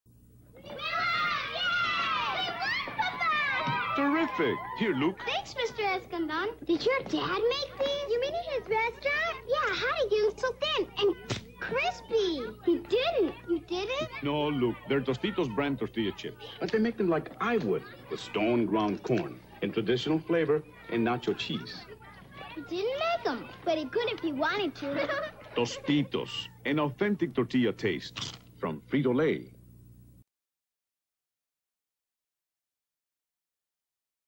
Vintage Tostitos Chips Commercial (1979)
"An authentic tortilla taste - from Frito-Lay."
Tostitos tortilla chips commercial featuring Traditional and Nacho Cheese flavors. The actor with #12 on his shirt is Rossie Harris ('Airplane'). Spot aired November 1979.